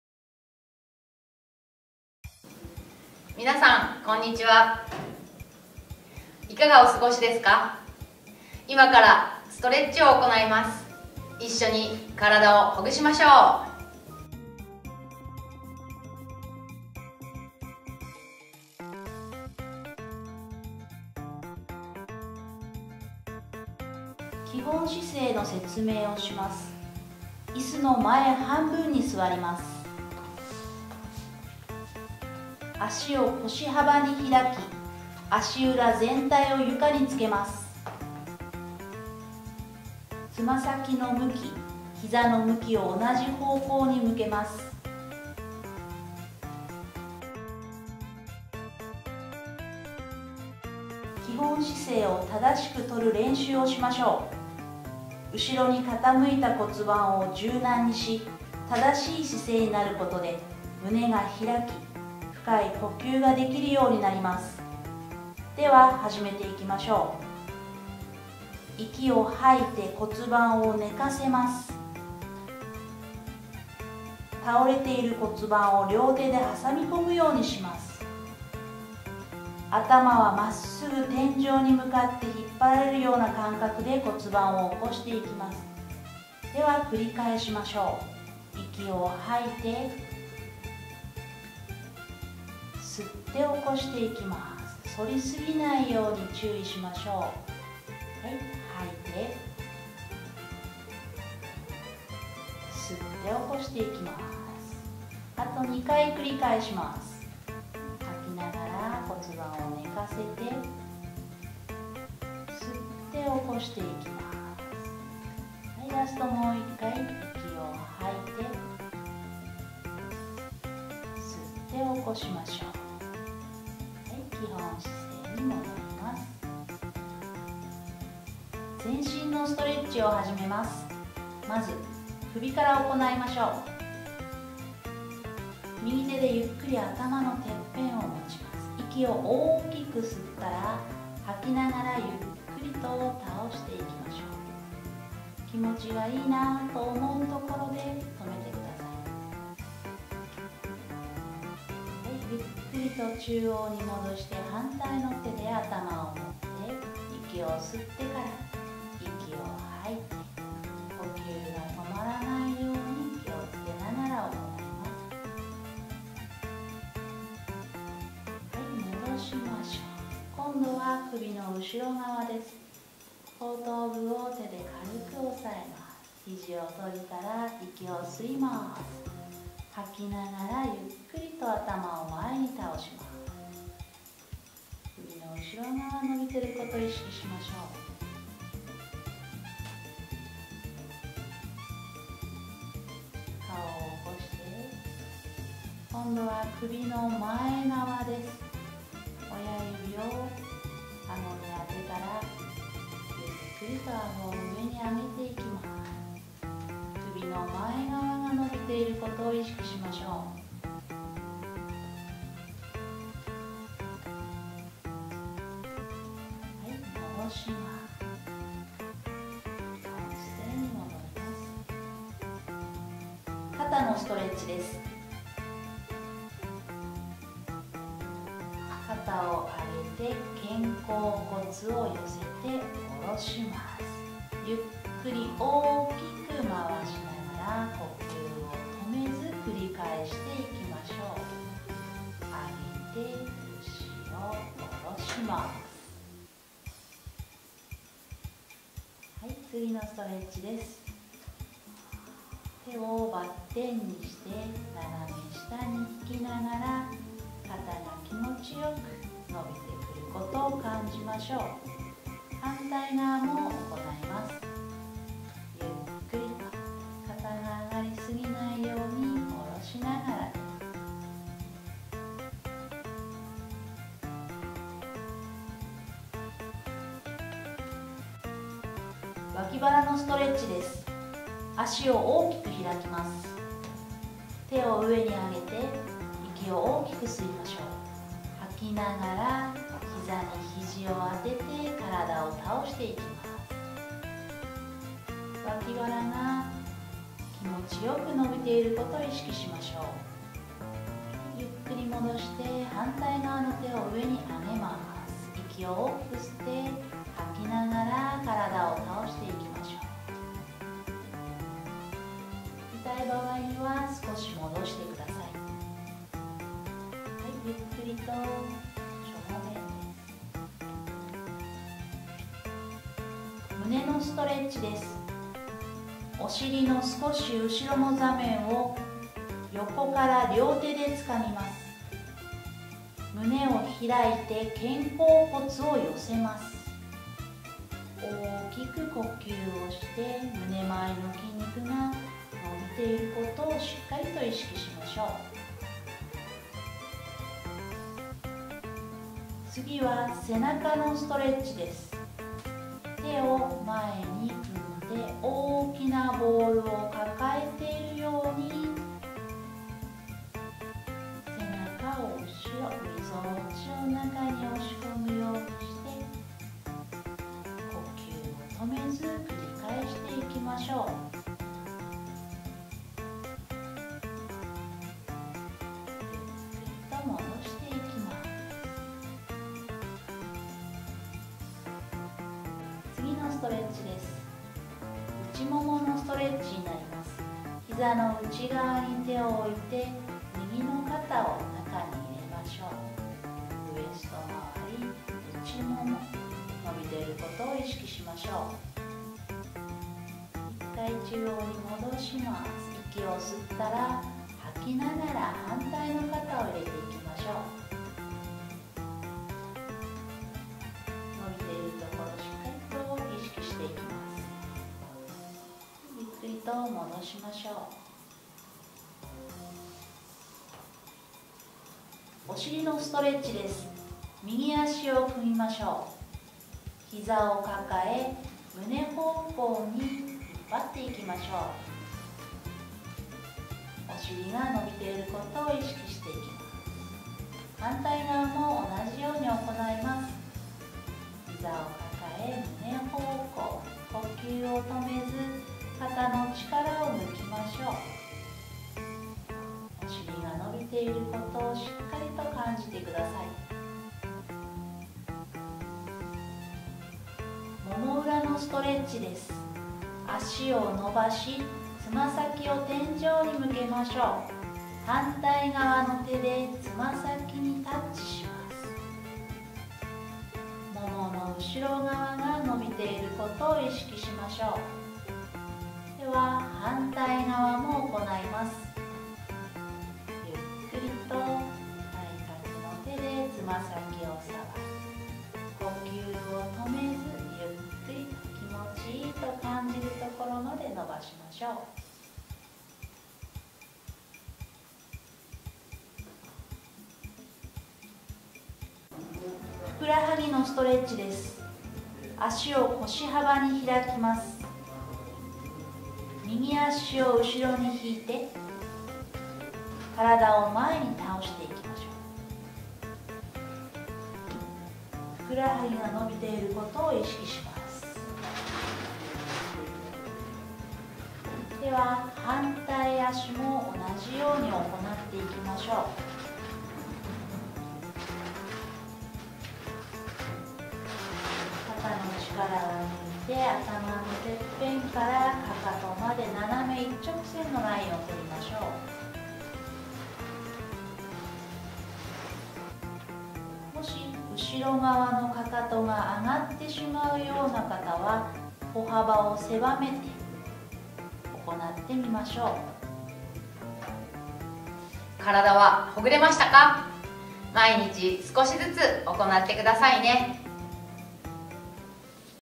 【茨城県常総市】おうち体操２～1日10分ストレッチ編～
おうち体操第２弾！
今回は身体をほぐすストレッチを行います。1日たったの10分で、肩こり・猫背・腰痛の予防にもなります。
毎日少しずつでも、続けて行うことが介護予防の第一歩です！